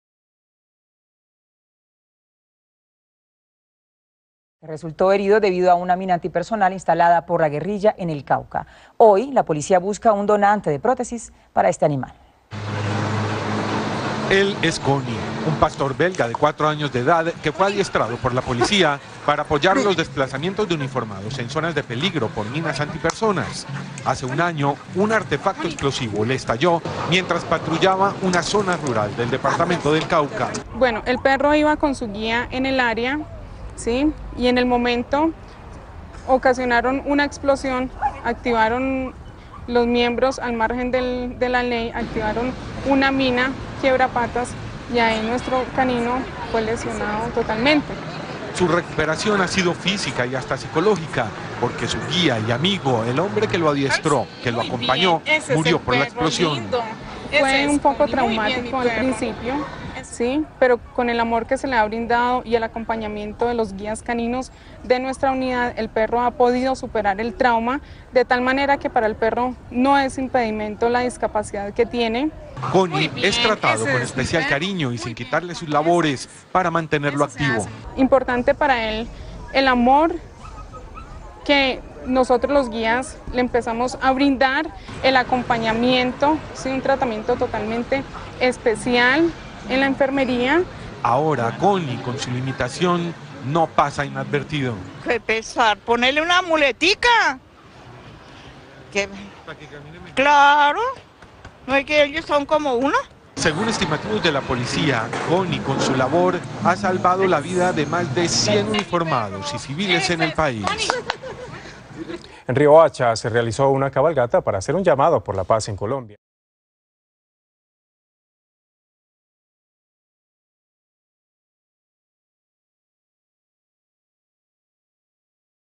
Buscan prótesis para perro antiexplosivos mutilado
El canino terminó afectado en un patrullaje en el Cauca.